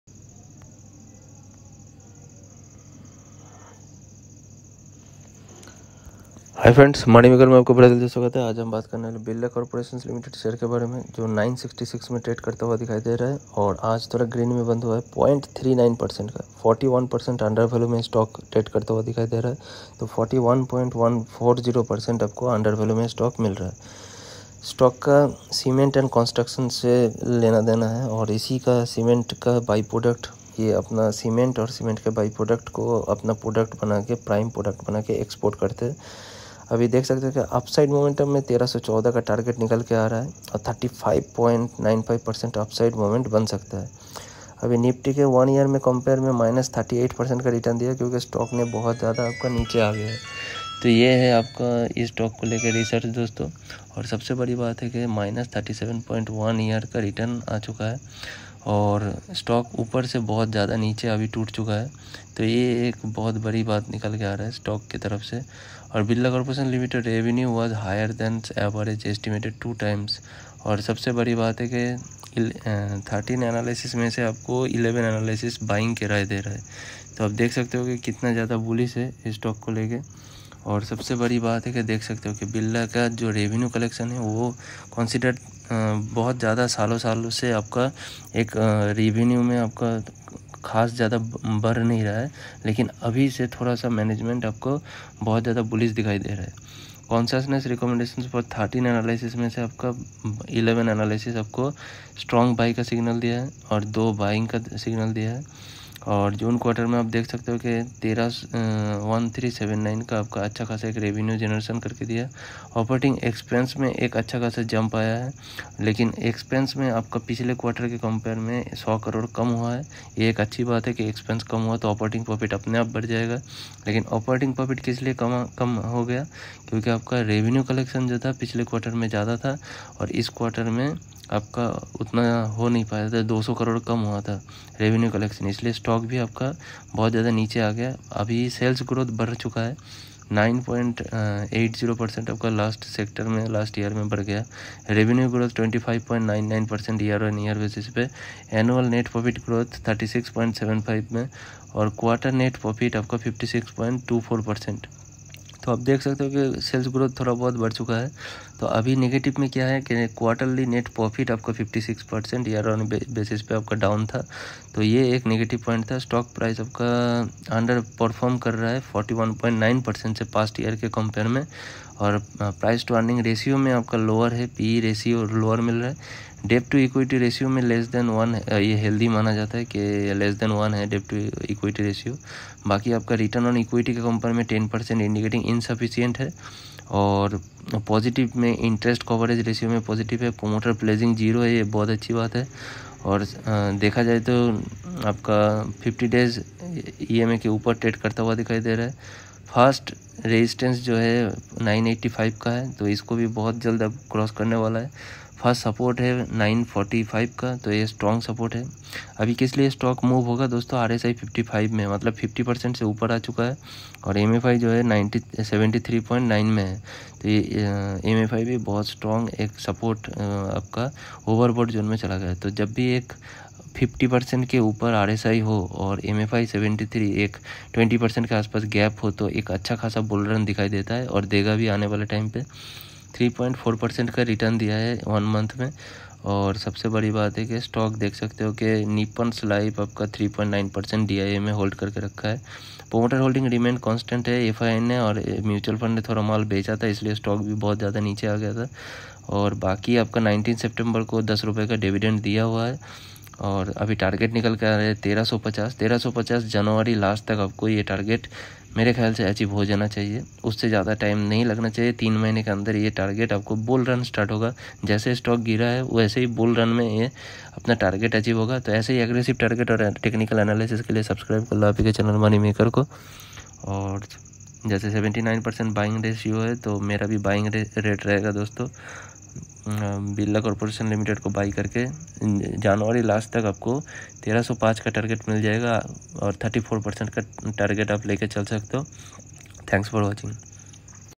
Birla corporation share latest technical analysis
Good fundamentals stock for short term investment opportunities but before investment please consult with your financial advisor